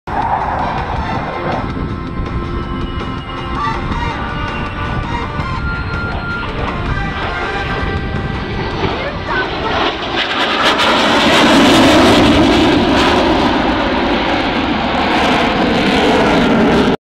F-18 low speed pass at Scott Air Show
Recorded on September 19, 2009, at Scott Air Force Base in Illinois.